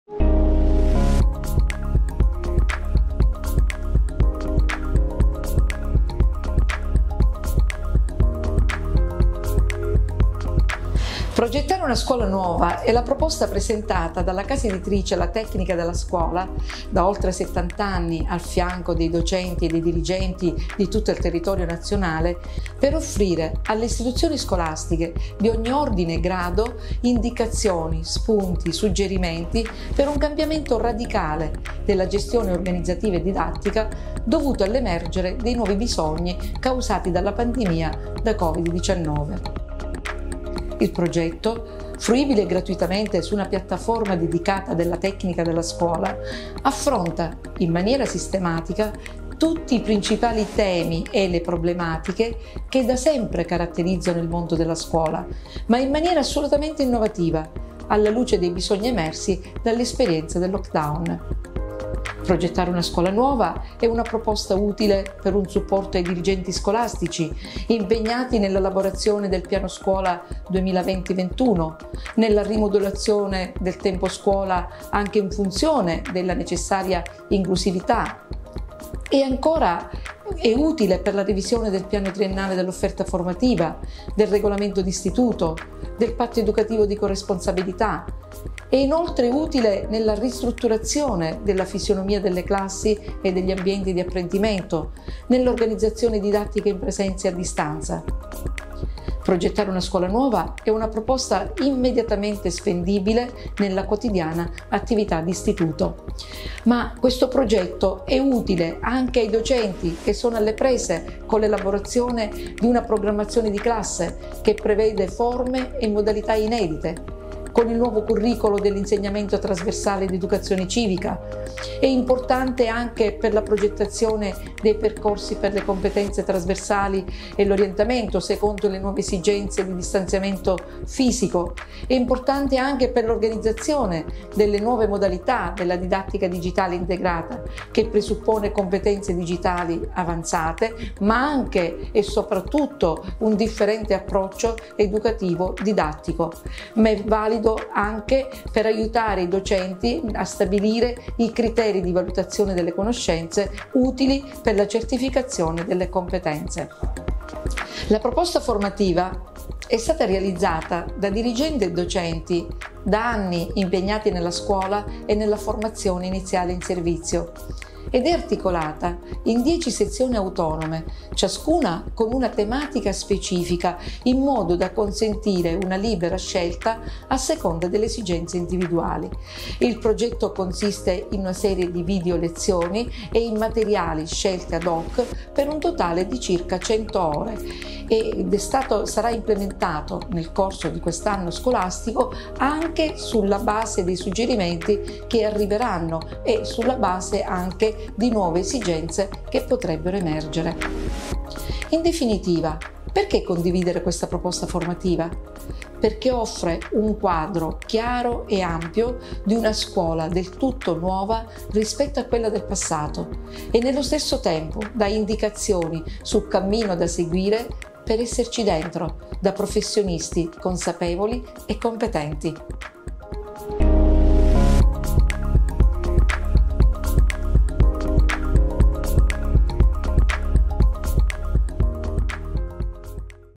Progettare una scuola nuova
Per sostenere docenti e dirigenti nel loro impegno quotidiano e per rispondere ai nuovi bisogni legati all'emergenza da Covid-19, La Tecnica della Scuola ha messo a disposizione la piattaforma digitale gratuita “Progettare una scuola nuova” con 100 ore di formazione tra video, materiali e altre risorse, suddivise in 12 sessioni formative.
Oltre questa data non sarà più possibile seguire i corsi gratuiti o scaricare i relativi attestati.